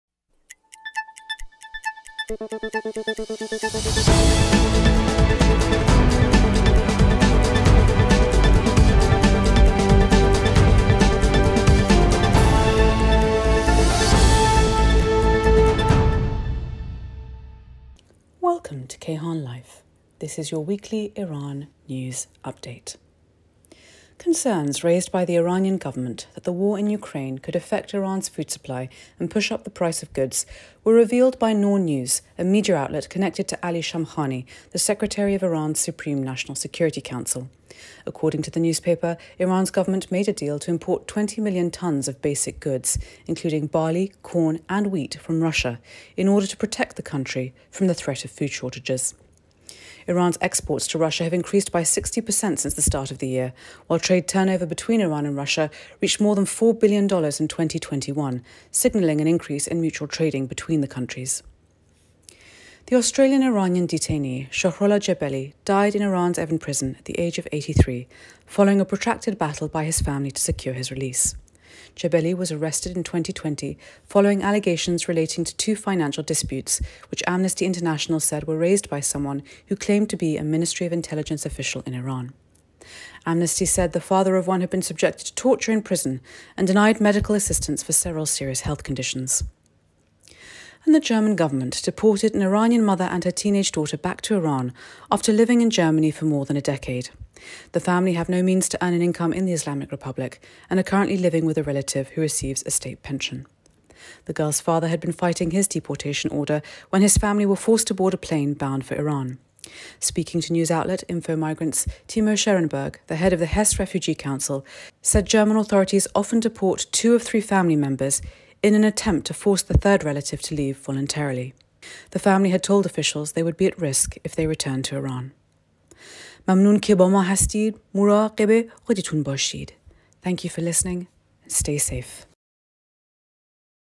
Iran News In Brief: March 25, 2022
Weekly podcasts offering a fast breakdown of Iran in the news.